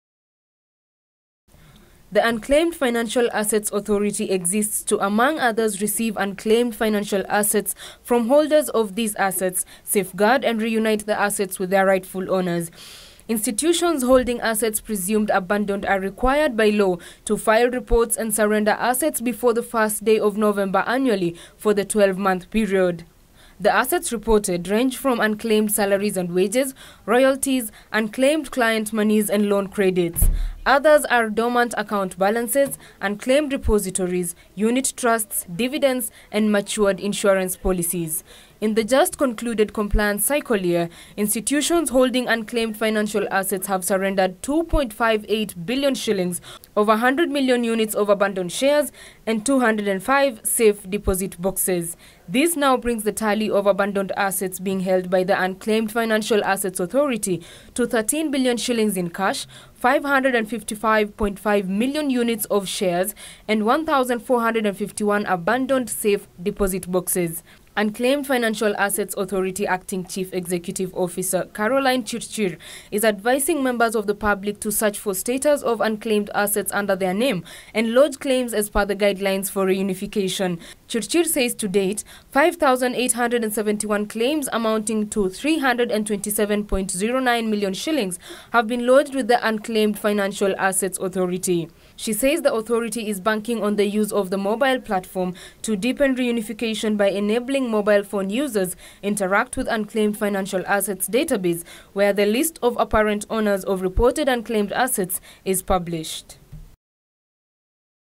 Unclaimed Financial Assets Authority holding 13 billion shillings
The Unclaimed Financial Assets Authority is holding 13 billion shillings in cash and 555.5 million units of shares that has to date been surrendered by institutions. This follows the submission of 2.58 billion shillings and over 100 million units of abandoned shares in the just concluded 2018 compliance cycle year.